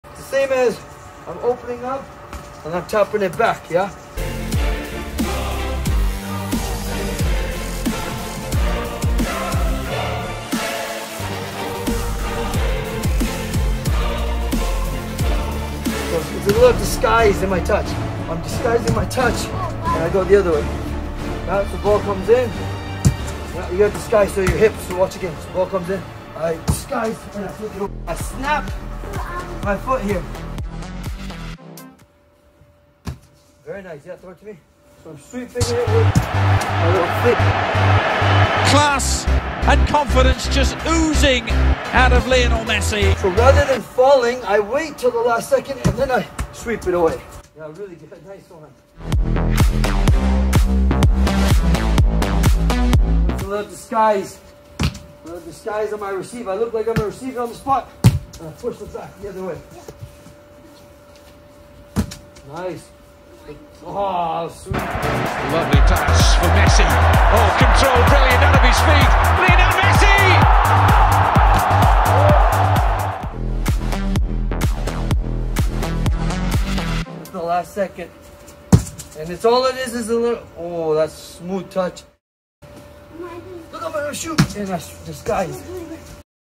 Tutorial on Aerial Receiving with a Disguised Touch With Inside of Foot
How to disguise your touch out of the air